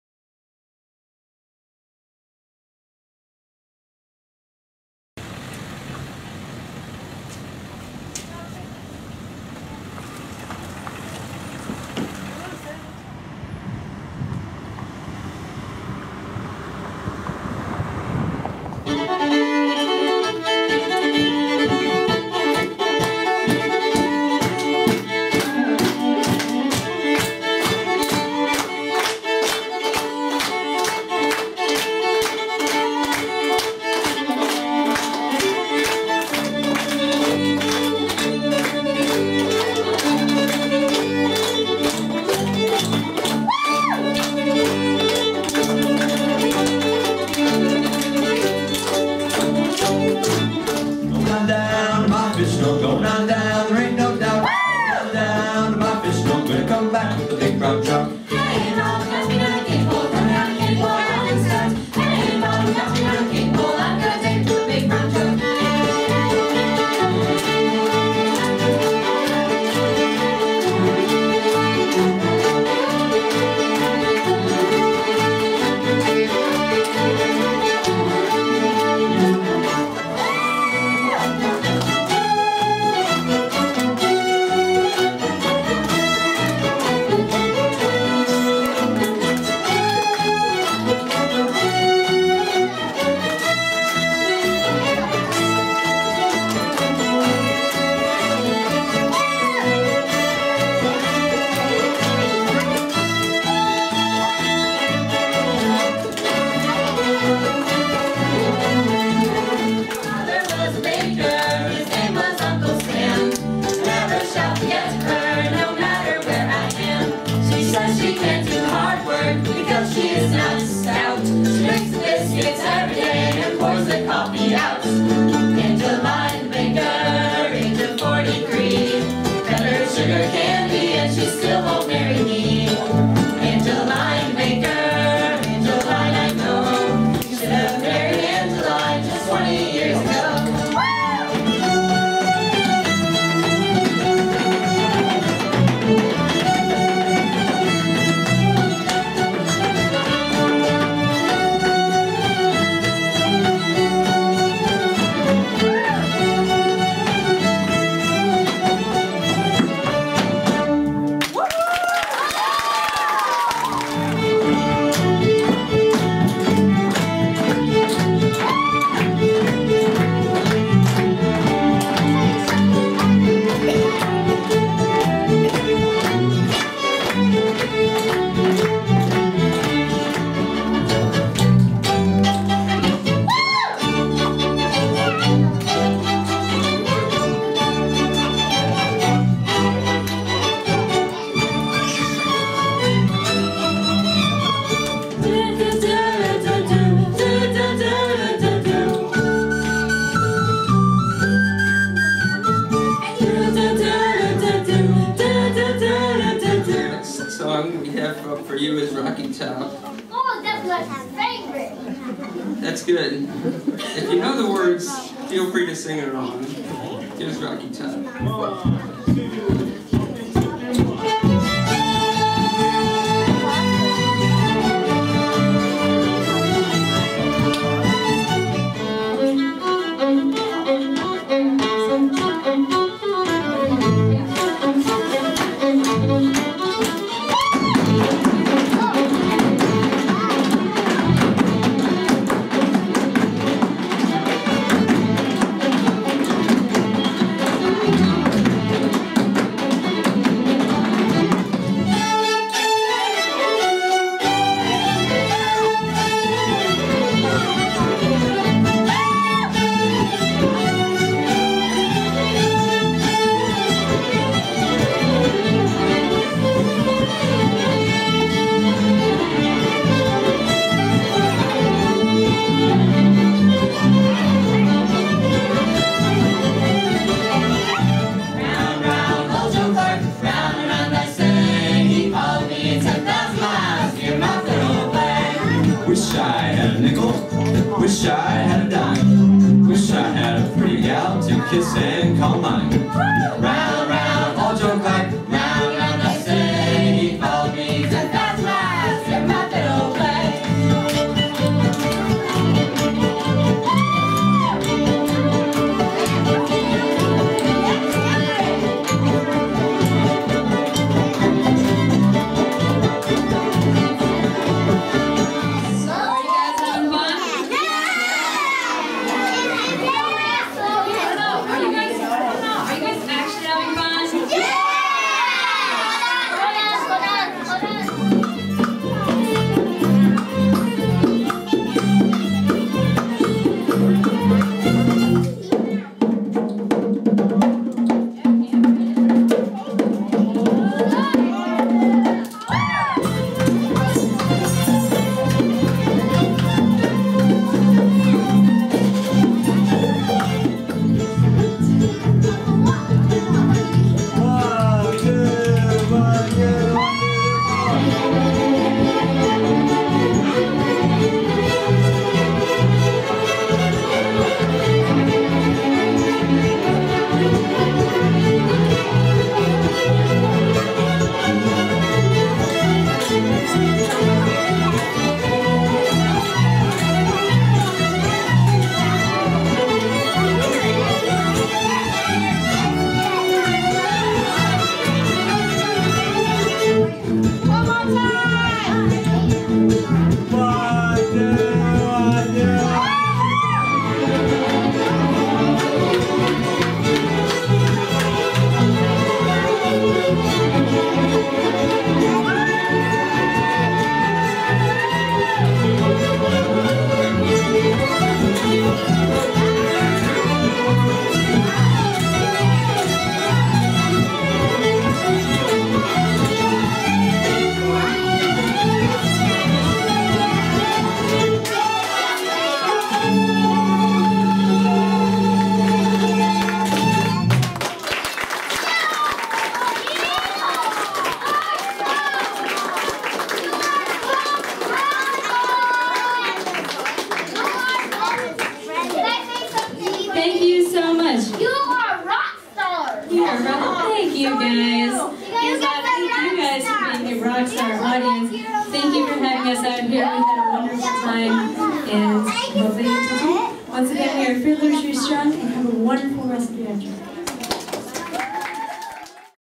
Fiddlers ReStrung 2013 - Short Clips - Veterans Memorial Library, City of Trenton, MI
Nothing is more fun than an audience of children!  Thank you for inviting us to play at your beautiful library!